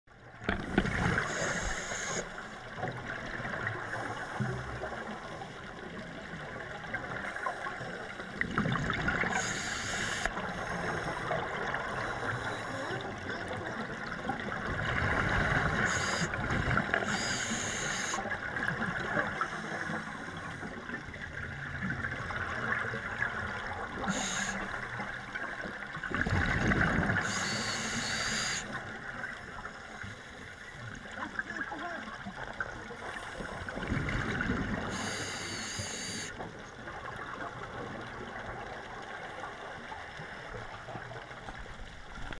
Playing with a Sea Cucumber GOPR0707
Playing catch with a Sea Cucumber at Veterans Park next to Redondo Beach Pier.